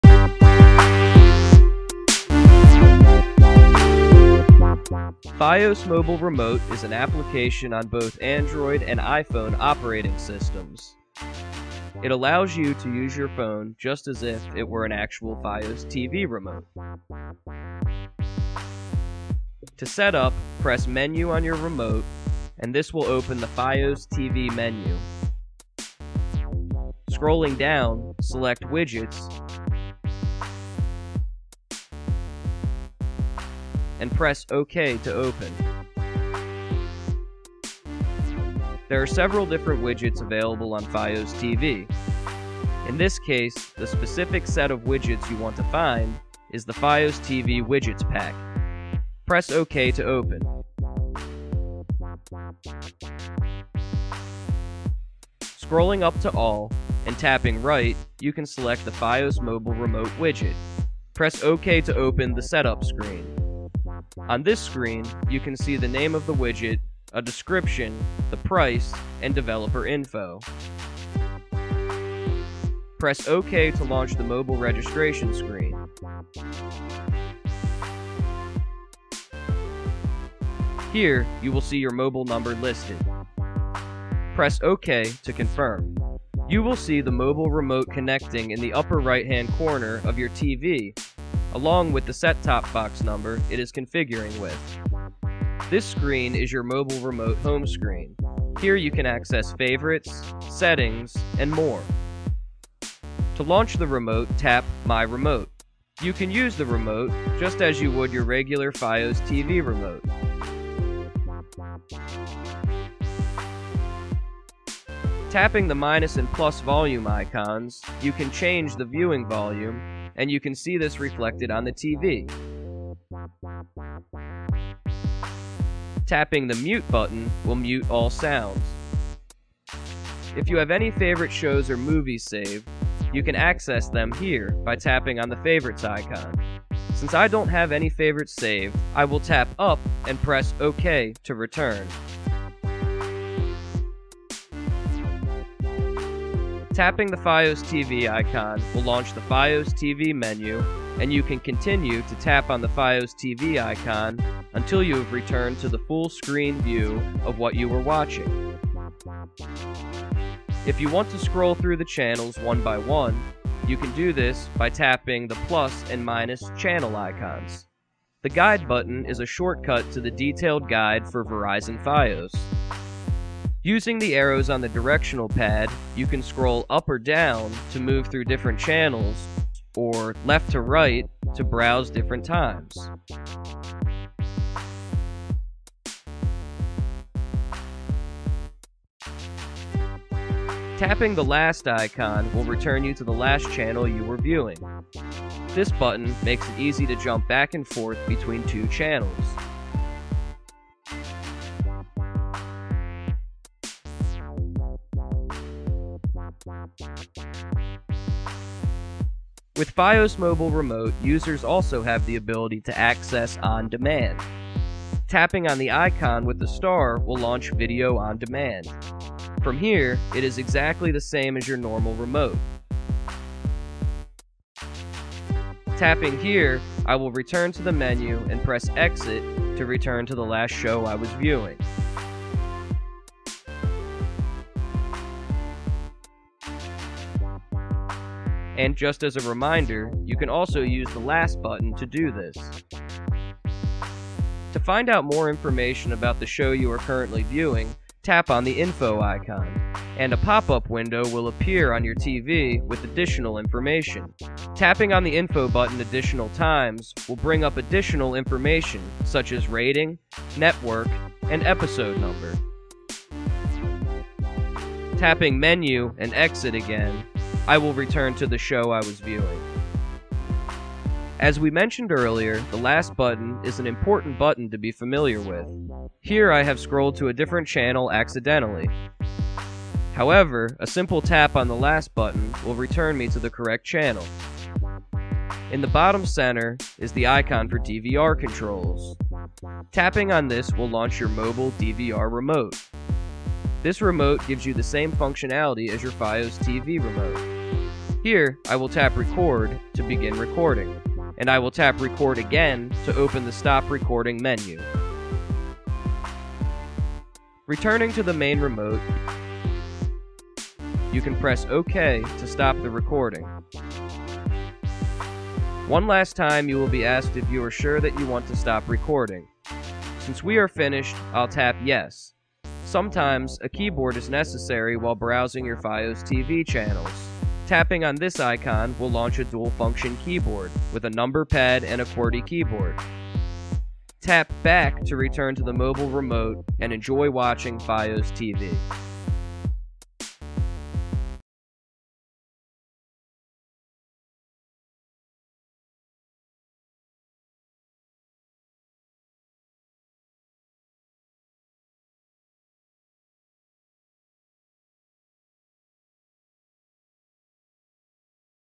Control FiOS with your phone!
In this tutorial we demonstrate how to setup and use FiOS Mobile Remote. Although demonstrated on an Android device this application and video is still applicable for iPhones.